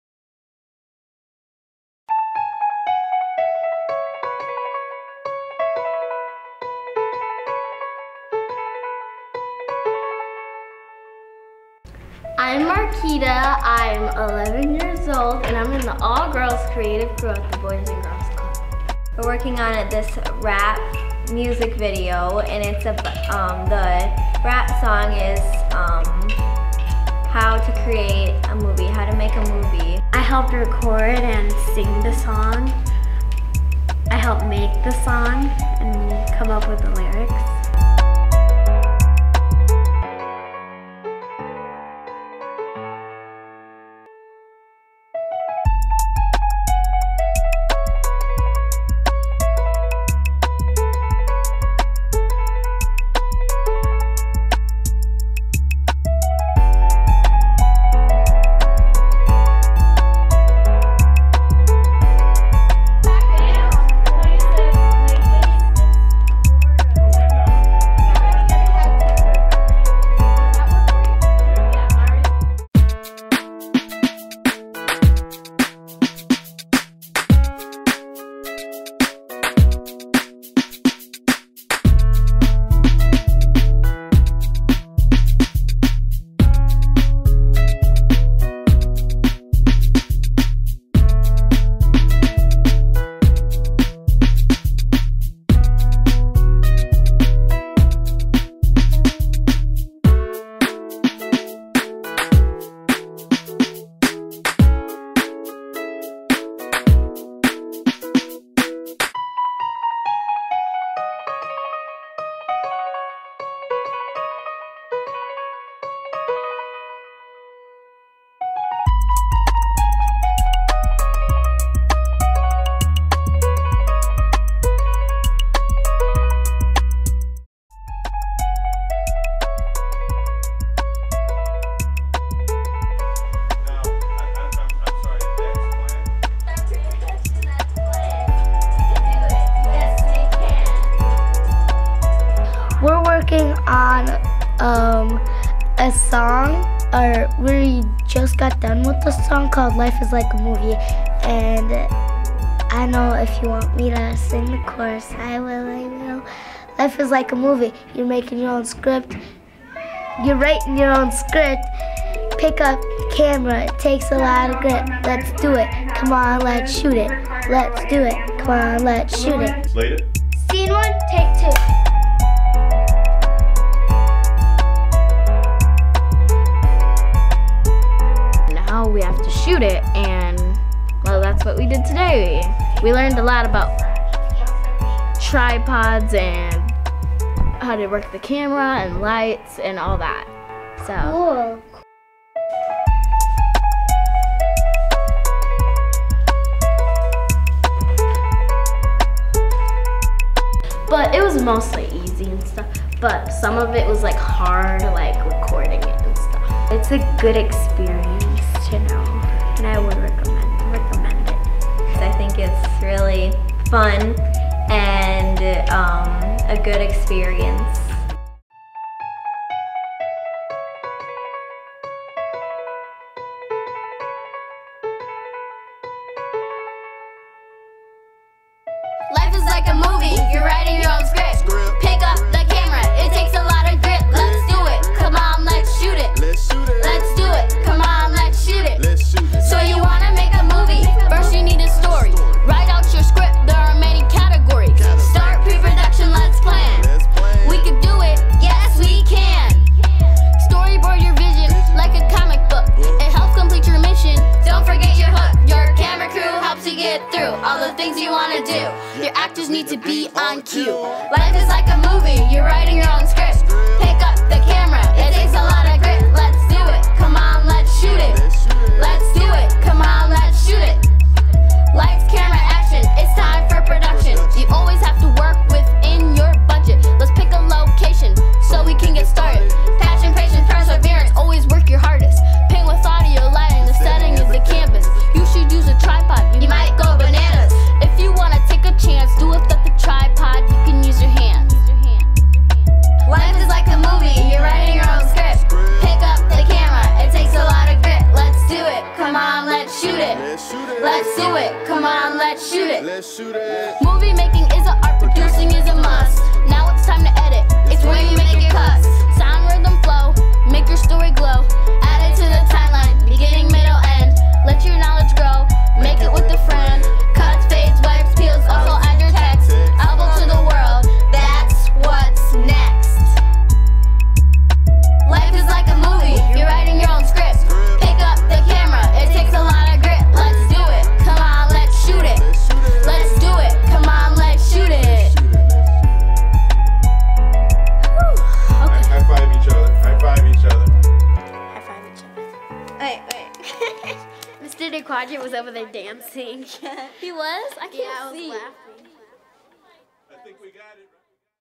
Making of Life Is Like a Movie - Boys and Girls Club Green Bay
The Boys & Girls Club of Greater Green Bay has been awarded a multi-year “Youth Arts Initiative” grant from The Wallace Foundation. This research based grant is an experiment to find out what it takes to implement an exceptionally high-level art programs in an after-school setting for 10 to 13 year-olds that would otherwise not have the opportunity.
The “Success Principles” being put to the test are derived from the Wallace Foundation Something to Say: Success Principles for Afterschool Arts Programs from Urban Youth and Other Experts research report. These principles were derived from a study of exemplary arts organizations serving youth, interviews with leading experts in arts education and youth development as well as interviews with Club Members and parents/guardians about what would make a successful high level arts program.
Support has allowed the Boys & Girls Club of Greater Green Bay to hire experienced, professional teaching artists to instruct skill development classes and equip facilities with top of the line technology and equipment. Through the Youth Arts Initiative, we have been able to implement four art forms/programs:

1) Graphic Arts: Visual Communication & Design
2) Dance: Choreography & Artistic Motion (Tap, Ballet & Jazz)
3) Digital Audio Arts: Music Production & Audio Engineering
4) Digital Visual Arts: Video Production & Editing

The Wallace Foundation has commissioned an independent evaluation of the Youth Arts Initiative from Research for Action aimed to develop evidence of outcomes and best practices to share with the youth development field on how organizations can implement high quality, impactful art programs.